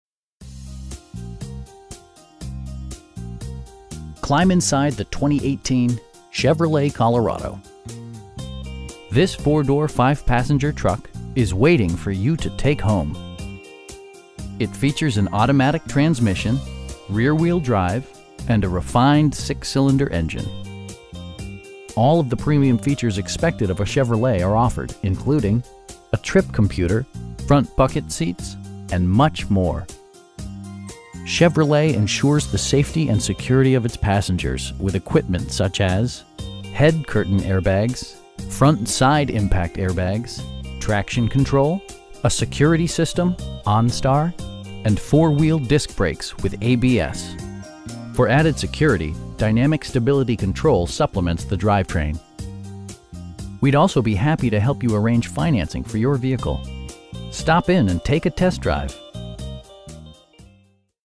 2018 Chevrolet Colorado 2WD LT in San Diego, CA 92111
Climb inside the 2018 Chevrolet Colorado. This 4 door, 5 passenger truck is waiting for you to take home! It features an automatic transmission, rear-wheel drive, and a refined 6 cylinder engine. All of the premium features expected of a Chevrolet are offered, including: a trip computer, front bucket seats, power door mirrors, power windows, cruise control, an overhead console, and much more. Chevrolet ensures the safety and security of its passengers with equipment such as: head curtain airbags, front side impact airbags, traction control, a security system, onStar, and 4 wheel disc brakes with ABS. For added security, dynamic Stability Control supplements the drivetrain. We'd also be happy to help you arrange financing for your vehicle. Stop in and take a test drive!